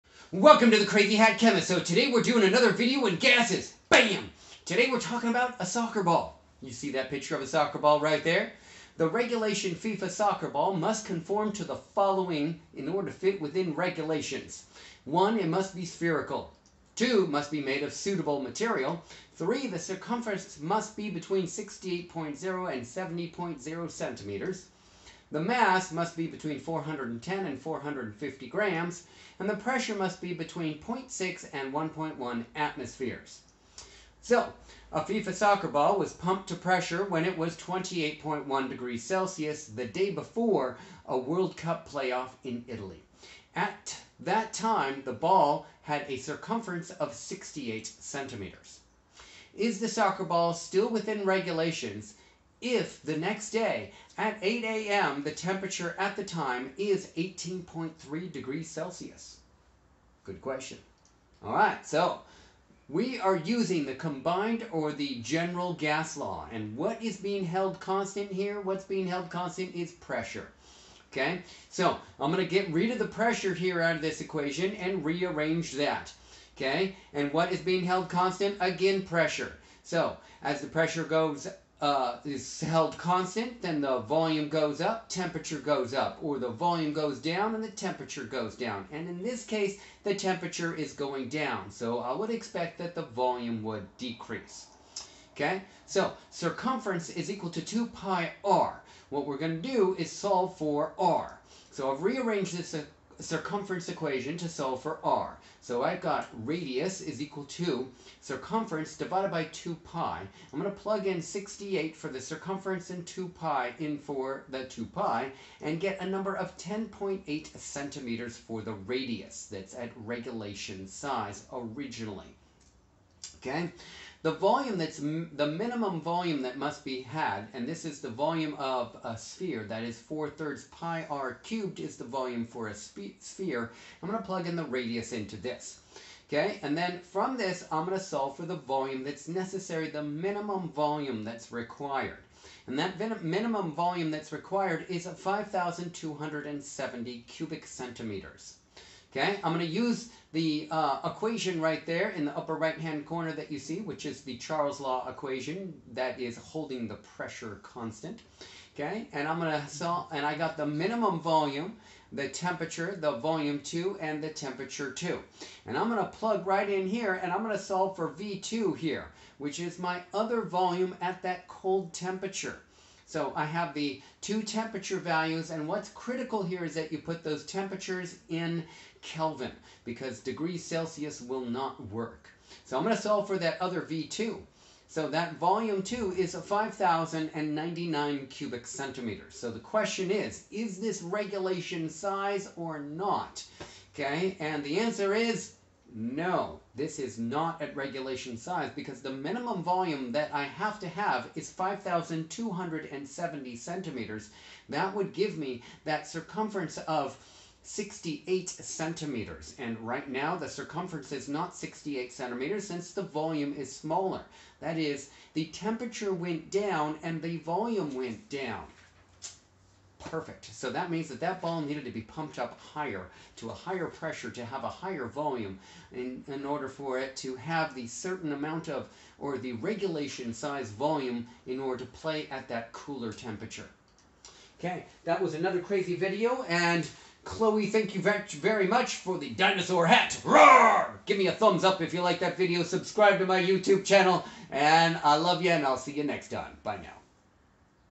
Gases 14: A Soccer Ball
An example of Charles' Law mathematically